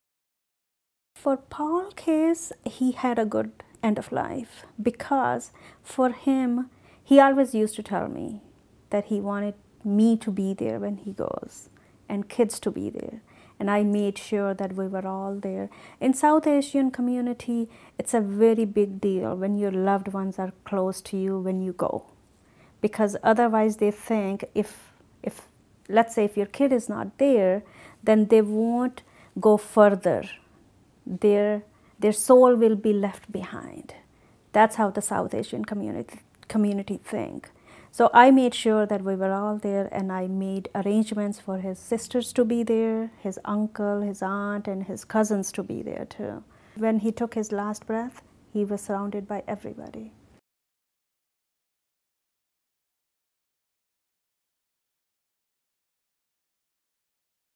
Kim: His last breath
In this video by Canadian Virtual Hospice, Kim explains what was important to her family to ensure a good death.